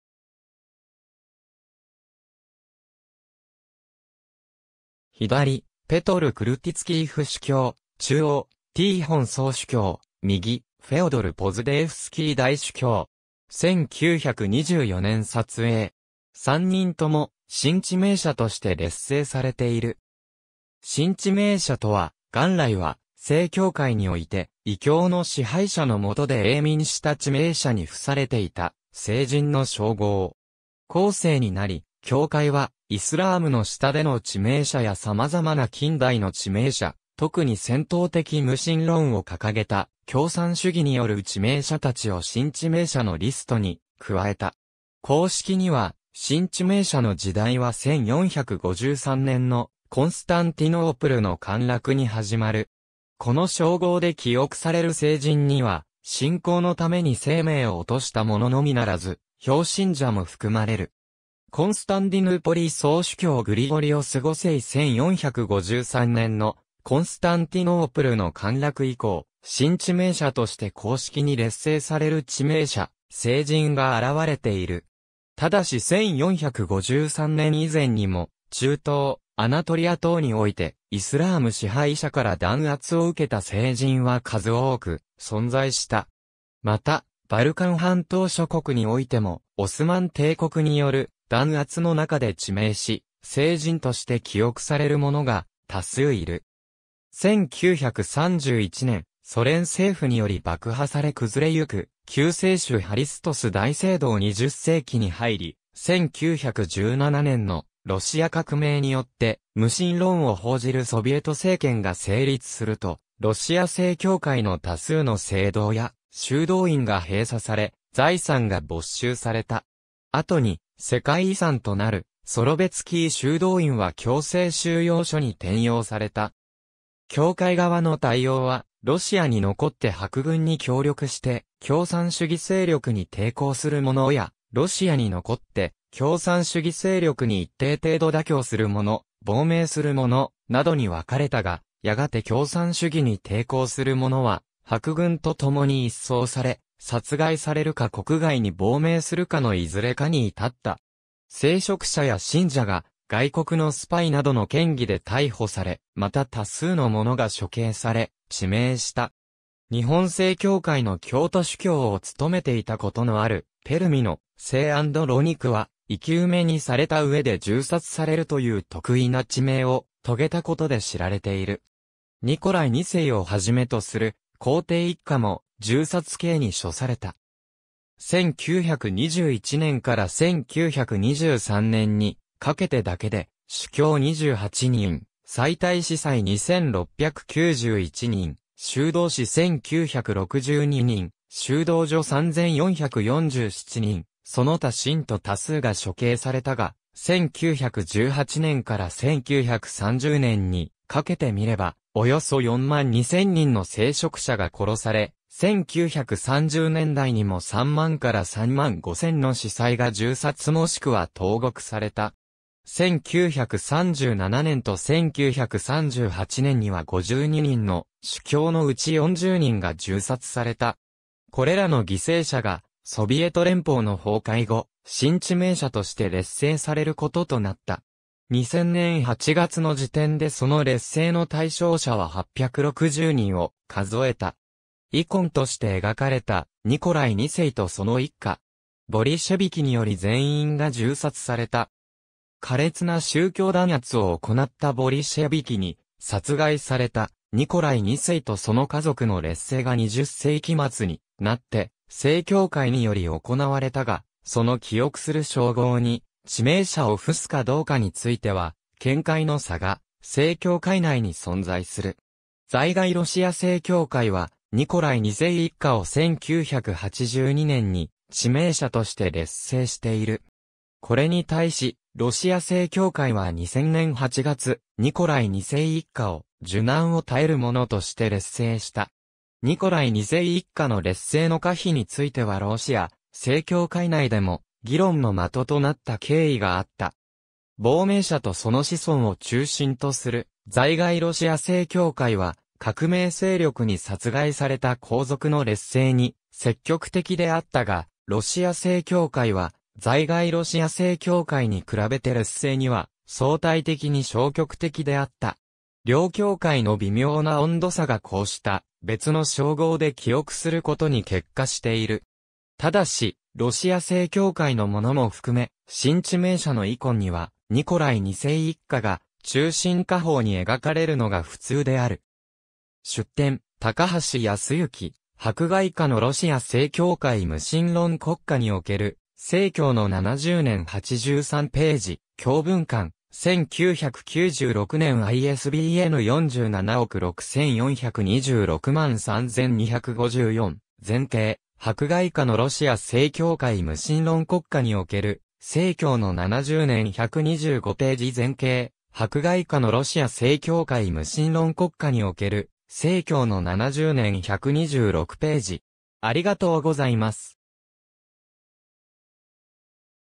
新致命者
左：ペトル・クルティツキイ府主教（1937年銃殺）、中央：ティーホン総主教（1925年病没）、右：フェオドル・ポズデエフスキイ大主教（1937年銃殺）。
1924年撮影。
3人とも新致命者として列聖されている。
新致命者（しんちめいしゃ、ギリシア語: Νεομάρτυρες, ロシア語: Новомученики, 英語: New Martyr）とは、元来は正教会において異教の支配者の下で永眠した致命者に付されていた聖人の称号。
後世になり、教会はイスラームの下での致命者や様々な近代の致命者、特に戦闘的無神論を掲げた共産主義による致命者達を新致命者のリストに加えた。
公式には、新致命者の時代は1453年のコンスタンティノープルの陥落に始まる。
この称号で記憶される聖人には、信仰のために生命を落としたもののみならず、表信者（信仰のために苦難を耐え忍んだ者）も含まれる。
コンスタンディヌーポリ総主教グリゴリオス5世 1453年のコンスタンティノープルの陥落以降、新致命者として公式に列聖される致命者・聖人が現れている。
ただし1453年以前にも、中東・アナトリア等において、イスラーム支配者から弾圧を受けた聖人は数多く存在した。
また、バルカン半島諸国においてもオスマン帝国による弾圧の中で致命し、聖人として記憶されるものが多数いる。
1931年、ソ連政府により爆破され崩れゆく、救世主ハリストス大聖堂 20世紀に入り、1917年のロシア革命によって無神論を奉じるソヴィエト政権が成立すると、ロシア正教会の多数の聖堂や修道院が閉鎖され、財産が没収された。
後に世界遺産となるソロヴェツキー修道院は強制収容所に転用された。
教会側の対応は、ロシアに残って白軍に協力して共産主義勢力に抵抗する者や、ロシアに残って共産主義勢力に一定程度妥協する者、亡命する者、などに分かれたが、やがて共産主義に抵抗する者は白軍とともに一掃され、殺害されるか国外に亡命するかのいずれかに至った。
聖職者や信者が外国のスパイなどの嫌疑で逮捕され、また多数の者が処刑され致命した。
日本正教会の京都主教を務めていたことのあるペルミの聖アンドロニクは、生き埋めにされた上で銃殺されるという特異な致命を遂げたことで知られている。
ニコライ2世をはじめとする皇帝一家も銃殺刑に処された。
1921年から1923年にかけてだけで、主教28人、妻帯司祭2691人、修道士1962人、修道女3447人、その他信徒多数が処刑されたが、1918年から1930年にかけてみれば、およそ4万2千人の聖職者が殺され、1930年代にも3万から3万5千の司祭が銃殺もしくは投獄された。
1937年と1938年には52人の主教のうち40人が銃殺された。
これらの犠牲者がソビエト連邦の崩壊後、新致命者として列聖されることとなった。
2000年8月の時点でその列聖の対象者は860人を数えた。
イコンとして描かれたニコライ2世とその一家。
ボリシェビキにより全員が銃殺された。
苛烈な宗教弾圧を行ったボリシェビキに殺害されたニコライ2世とその家族の列聖が20世紀末になって正教会により行われたが、その記憶する称号に「致命者」を付すかどうかについては見解の差が正教会内に存在する。
在外ロシア正教会はニコライ2世一家を1982年に致命者（Martyrs）として列聖している。
これに対し、ロシア正教会は2000年8月、ニコライ2世一家を「受難を耐える者（ストラストチェールペツ、Passion-Bearers）」として列聖した。
ニコライ2世一家の列聖の可否についてはロシア正教会内でも議論の的となった経緯があった。
亡命者とその子孫を中心とする在外ロシア正教会は革命勢力に殺害された皇族の列聖に積極的であったが、ロシア正教会は在外ロシア正教会に比べて列聖には相対的に消極的であった。
両教会の微妙な温度差がこうした別の称号で記憶することに結果している。
ただし、ロシア正教会のものも含め、新致命者のイコンにはニコライ2世一家が中心下方に描かれるのが普通である。
^ 出典：高橋保行『迫害下のロシア正教会　無神論国家における正教の70年』83頁、教文館、1996年　ISBN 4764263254 ^ 前掲『迫害下のロシア正教会　無神論国家における正教の70年』125頁 ^ 前掲『迫害下のロシア正教会　無神論国家における正教の70年』126頁